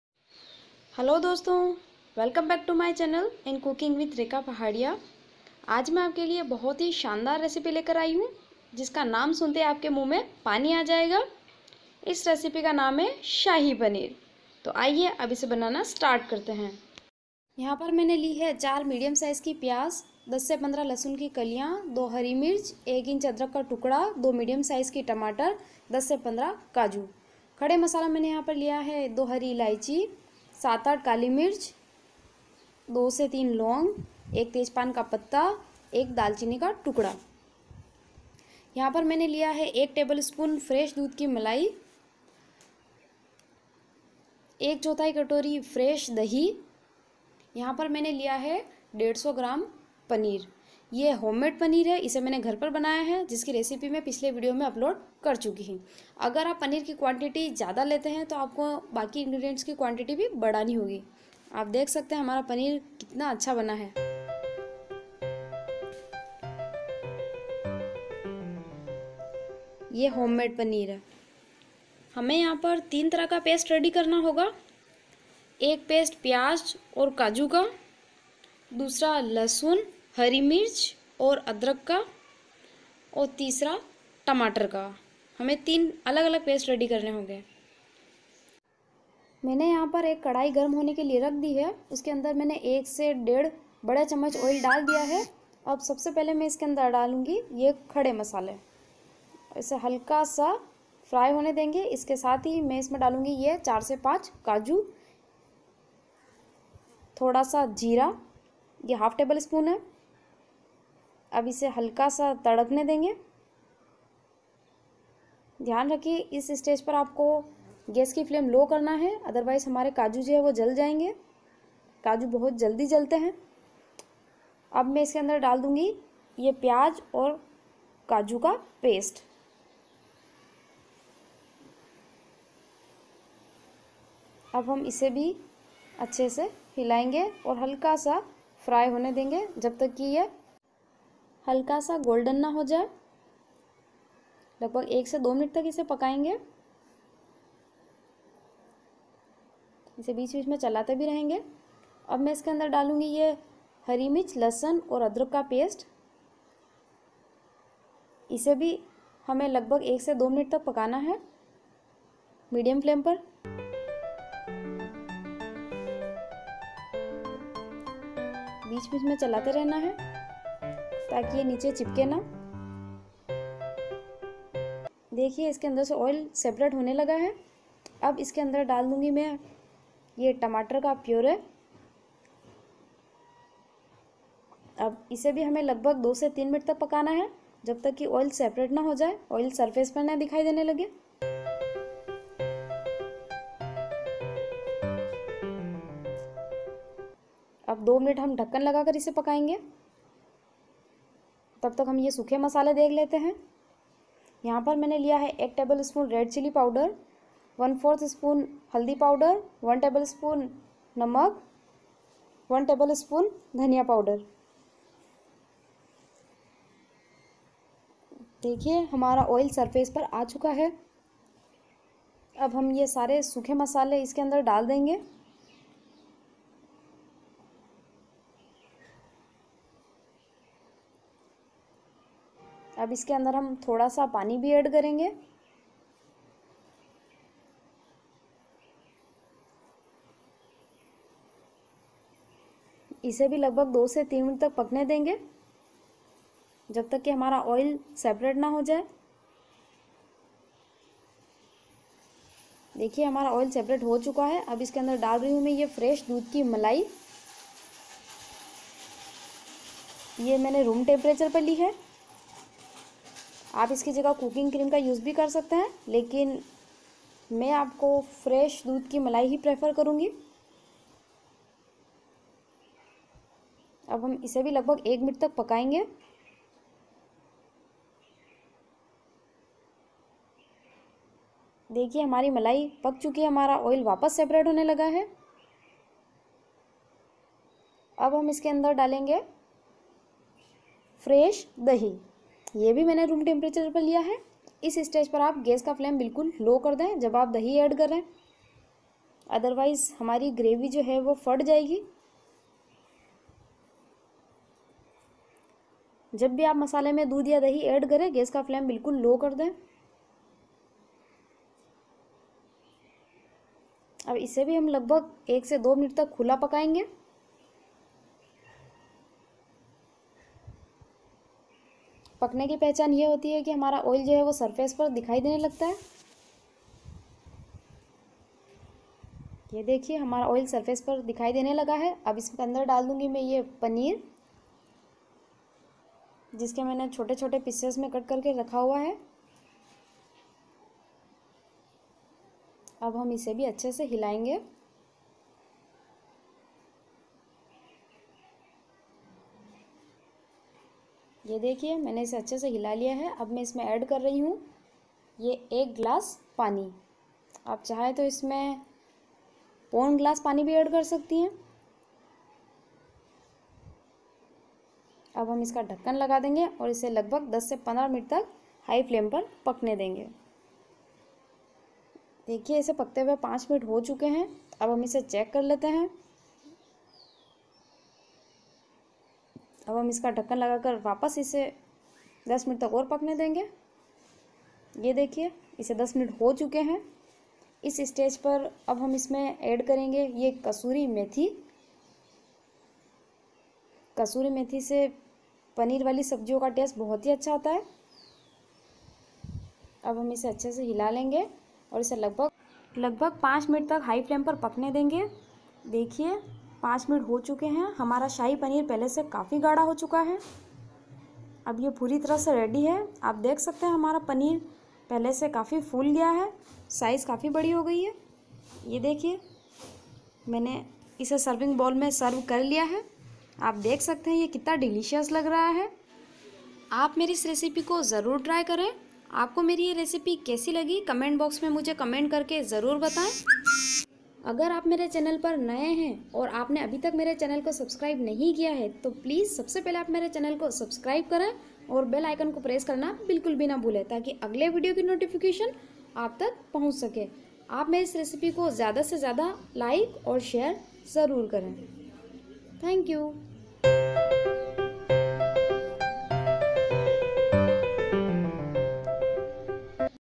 अगर एक बार आप ये घर का बना शाही पनीर खायेंगे तो रेस्टॉरेंट का खाना भूल जाएंगे । Shahi Paneer Recipe।
Is video me mene apko bhut hi easy tarike se btaya h ki ghar par bhut hi aasani se ap shadi party wala ya restaurant style shahi paneer kese bana sakte hai

Ingredients of shahi paneer-

Oil
Paneer(150gm)
Onion(4 medium)
Tomato(2-3 medium)
Garlic buds(lahsun ki kaliyan10-12)
Cashew(10-12)

Cumin seeds(jeera)
Cloves(laung)
Black pepper(kali mirch)
Cinnamon(dalchini)
Bay leaves(tejpan ka patta)

Turmeric powder(1/4tsp)
Salt(1tsp)
Garam masala powder (optional)
Coriander powder(1tsp)

Curd(1/4 bowl dahi fresh)
Fresh milk cream (1tsp)
Fenugreek seeds(kasturi methi)(1/2tsp)